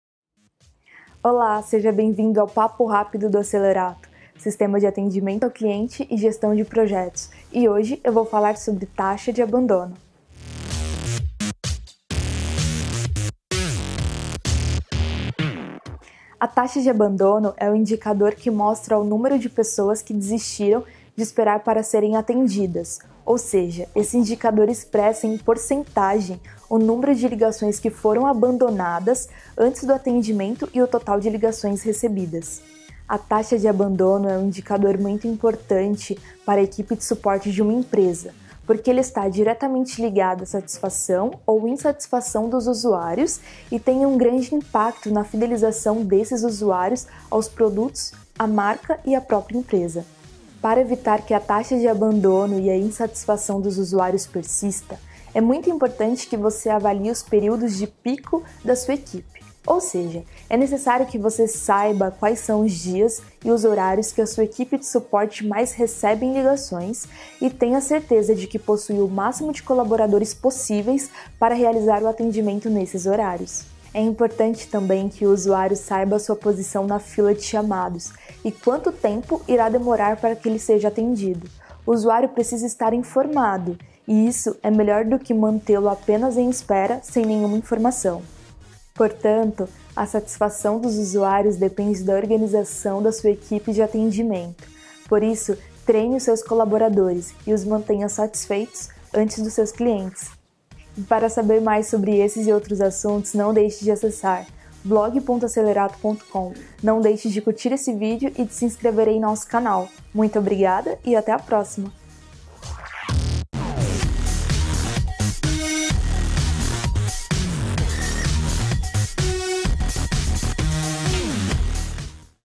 Taxa de Abandono | Papo Rápido | T4E6
Neste Papo Rápido do Acelerato, Sistema de Atendimento ao Cliente e Gestão de Projetos, Joyce Souza fala sobre Taxa de Abandono.

A “Taxa de Abandono” é o indicador que mostra o número de pessoas que desistiram de esperar para serem atendidas, ou seja, esse indicador expressa em porcentagem o número de ligações que foram abandonadas antes do atendimento e o total de ligações recebidas.

A taxa de abandono é um indicador muito importante para a equipe de suporte de uma empresa porque ela está diretamente ligada a satisfação ou insatisfação dos usuários e tem um grande impacto na fidelização desses usuários aos produtos, a marca e a própria empresa.

Para evitar que a “taxa de abandono” e a insatisfação dos usuários persista, é muito importante que você avalie os períodos de pico da sua equipe, ou seja, é necessário que você saiba quais são os dias e os horários que a sua equipe de suporte mais recebe ligações e tenha a certeza de que possui o máximo de colaboradores possíveis para realizar o atendimento nestes momentos.

É importante também que o usuário saiba a sua posição na fila de chamados e quanto tempo demorará para que ele seja atendido. O usuário precisa estar informado e isso é melhor do que mantê-lo apenas na espera sem nenhuma informação.

Portanto, a satisfação dos usuários depende da organização da equipe para o atendimento, por isso, treine os seus colaboradores e os mantenha satisfeitos antes dos seus clientes.

Este Papo Rápido é um oferecimento de Acelerato - Gestão de Projetos ágeis e Help Desk
Acesse e descubra como podemos ajudá-lo.